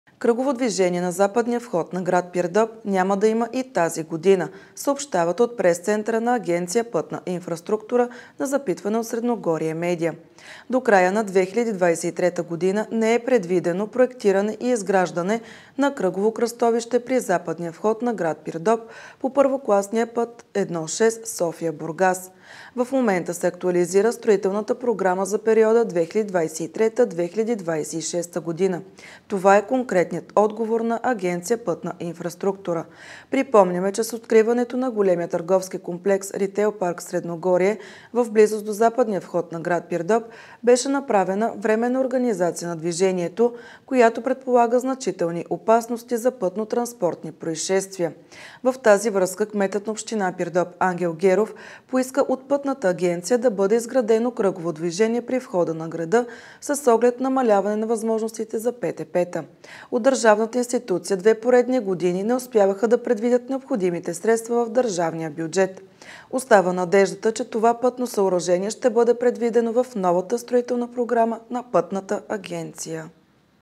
Кръгово движение при Пирдоп не е предвидено до края на 2023г.
Кръгово движение на западния вход на гр.Пирдоп няма да има и тази година, съобщават от пресцентъра на Агенция „Пътна инфраструктура” /АПИ/ на запитване от „Средногорие Медия“.
До края на 2023 година не е предвидено проектиране и изграждане на кръгово кръстовище при западния вход на град Пирдоп по първокласния път I-6 София – Бургас. В момента се актуализира строителната програма за периода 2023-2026 г. Това е конкретния отговор на АПИ.
Припомняме, че с откриването на големия търговски комплекс „Ритейл парк Средногорие“ в близост до западния вход на гр.Пирдоп беше направена временна организация на движението, която предполага значителни опасности за пътно-транспортни произшествия /ПТП/. В тази връзка кметът на община Пирдоп Ангел Геров поиска от пътната агенция да бъде изградено кръгово движение при входа на града с оглед намаляване на възможните ПТП-та. От държавната институция две поредни години не успяваха да предвидят необходимите средства в държавния бюджет.
Остава надеждата, че това пътно съоръжение ще бъде предвидено в новата строителна програма на пътната агенция.